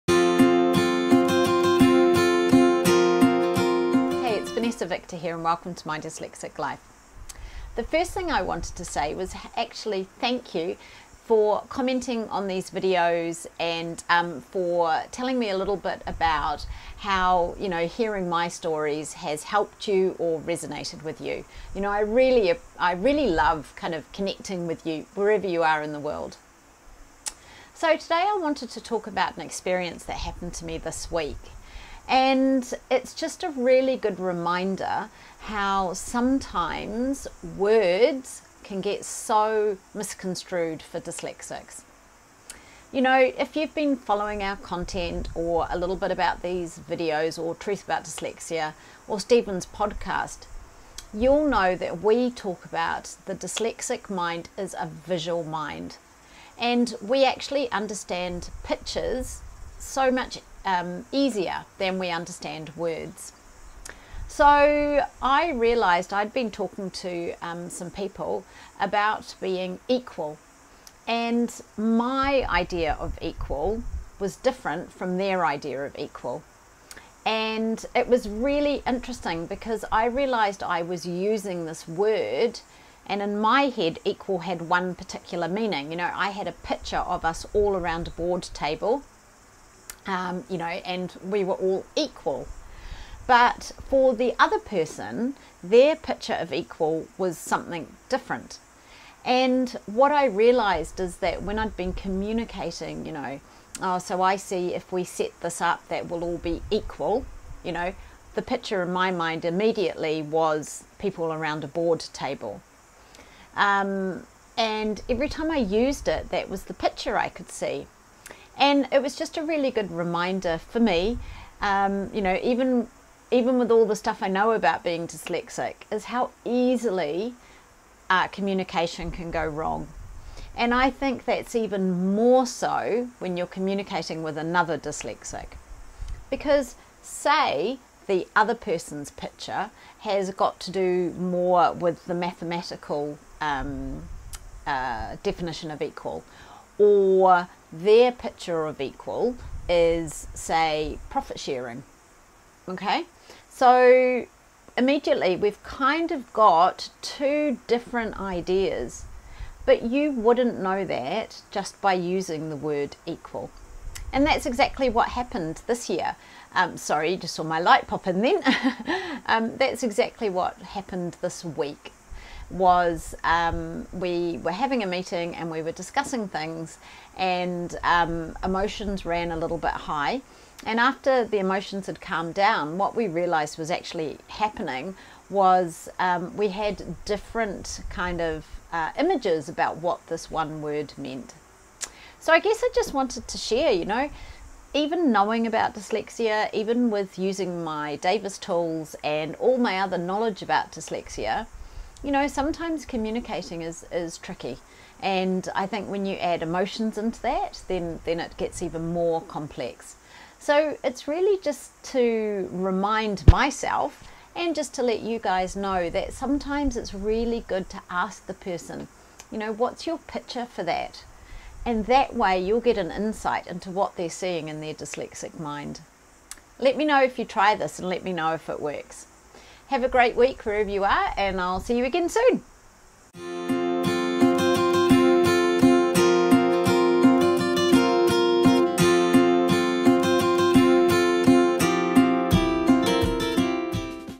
Communication: A dyslexic minefield
As dyslexics we do not naturally think with words!

So when we are communicating with people it can really cause problems if we have a different idea/feeling or even visual image of what a word may mean.

I was reminded of this during the week.

We were in a meeting and I couldn’t get why the others weren’t as enthusiastic or as on board with my idea. In fact they seemed to be a bit resentful, I was so blindsided by this.

After the emotions had calmed down I realised that they had a different feeling and picture for the word equal.

It really reminded me that the best thing we can say to a dyslexic is, “what is your picture for that?” Or “what is the feeling attached to that word?”.

This way you can find out at the start if you have different images/ideas or feelings!

Dyslexia is so much more than just reading, writing and spelling. Find out more on our podcast & in our private group below.